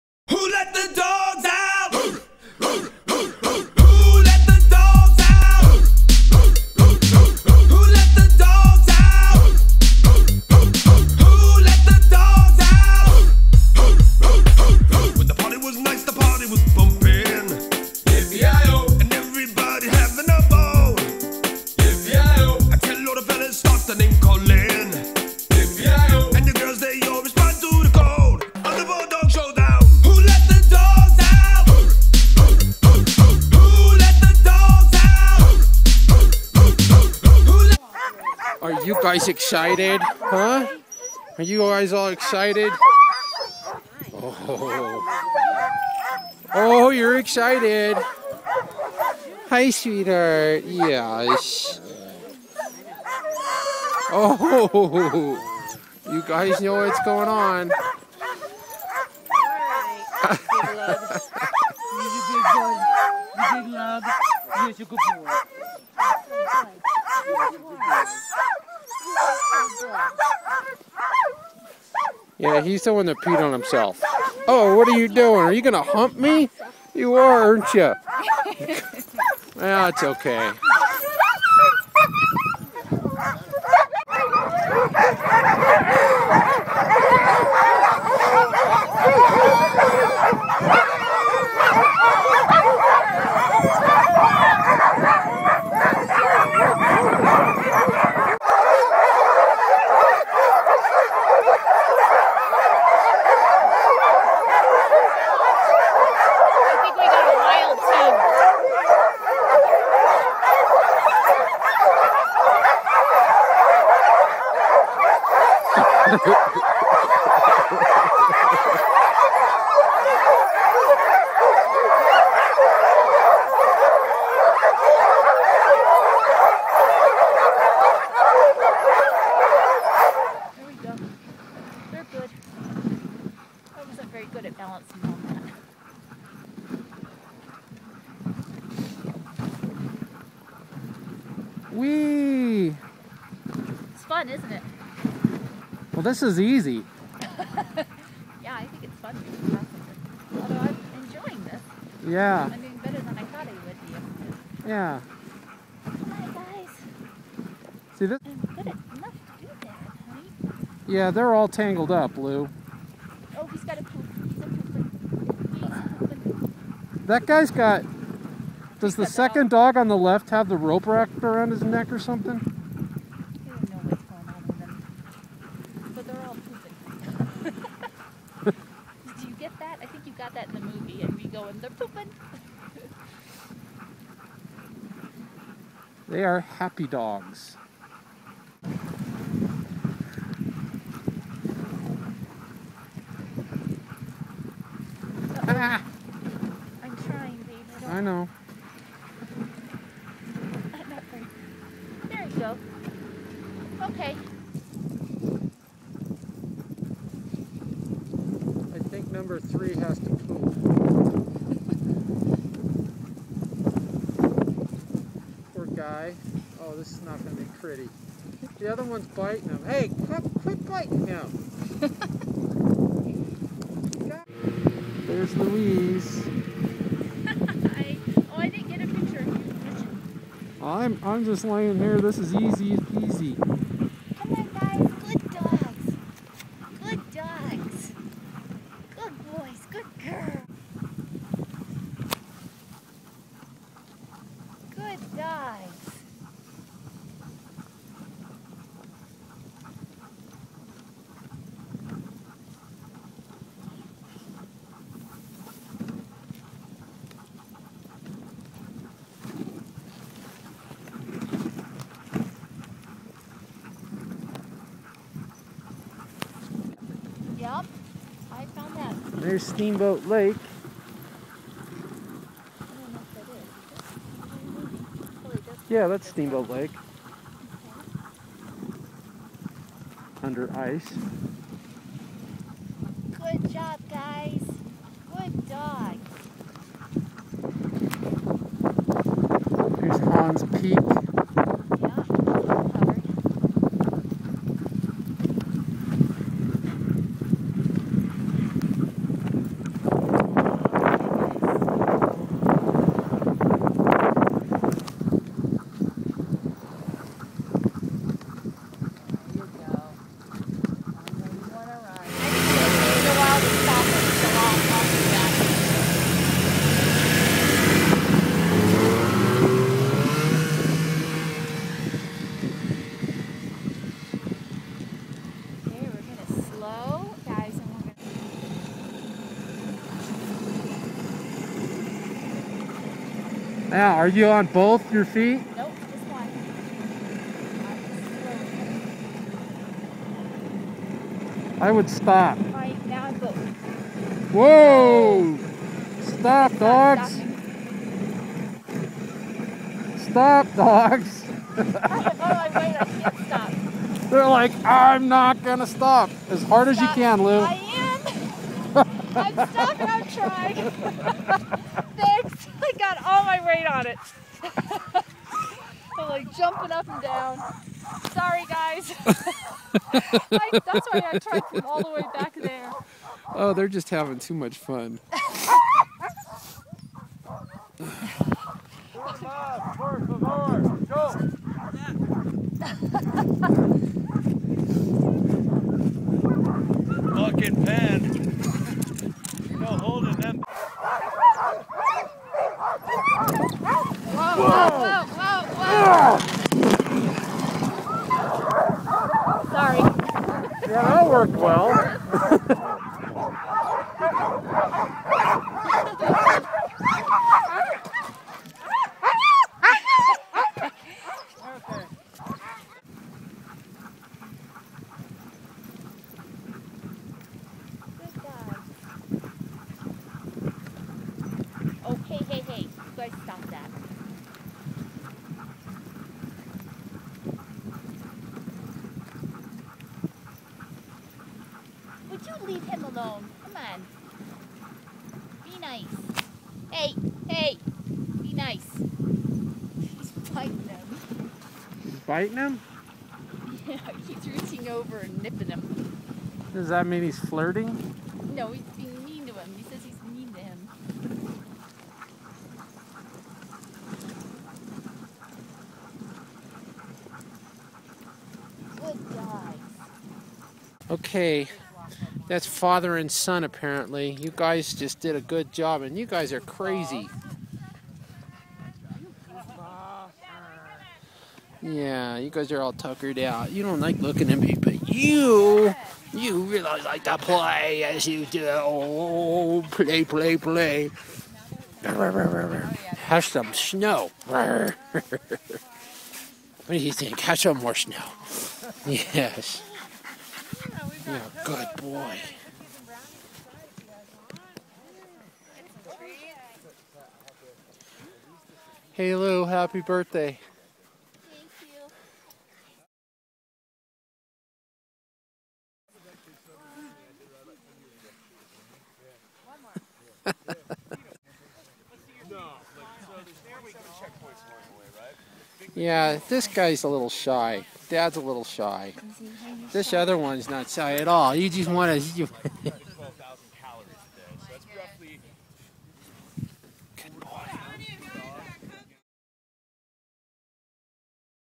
Dogsled
A Christmas gift from her parents, we went on a half day dogsled tour.   Usually held down in Milner, but the snow has not been great down in the valley early 2012, so Steamboat Lake is utilized at a much higher elevation.  These dogs have 1 speed.  Fast as they can go!  So much fun, these dogs have contagious enthusiasm.